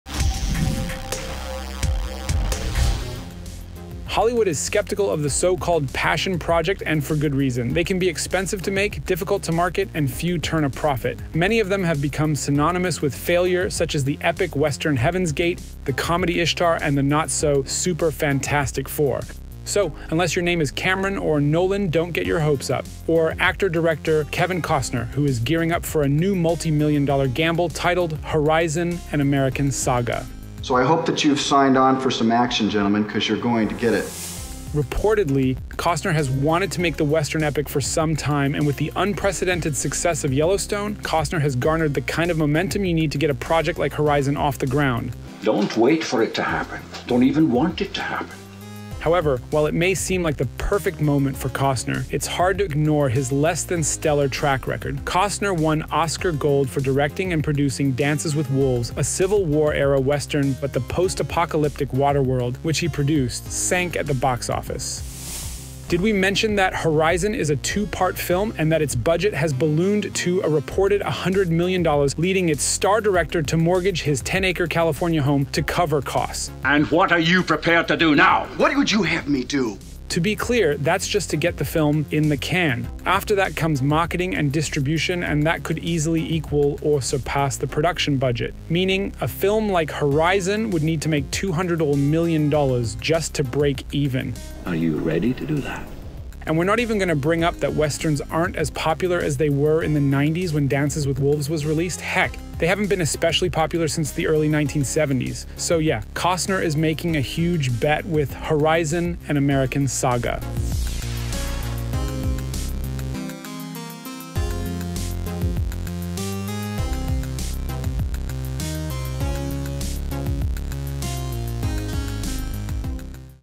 Horizon Repeats A Multi-Million Dollar Kevin Costner Gamble For The 15th Time In 33 Years
Set for a 2024 release, the two-part Horizon is one of Kevin Costner's most ambitious risks yet—and he's no stranger to multi-million dollar gambles.

Script by: Kate Bove, G. Ducat

Voice Over by: G. Ducat

Edited by: G. Ducat